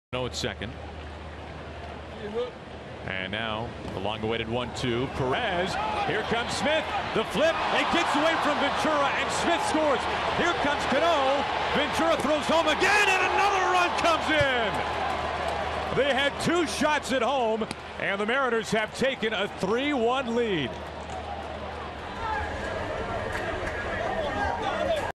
SEA@KC: Wild pitch, error allows two to score
With Nelson Cruz at the plate, a wild pitch from Yordano Ventura allows Seth Smith to score, then Robinson Cano comes home on an error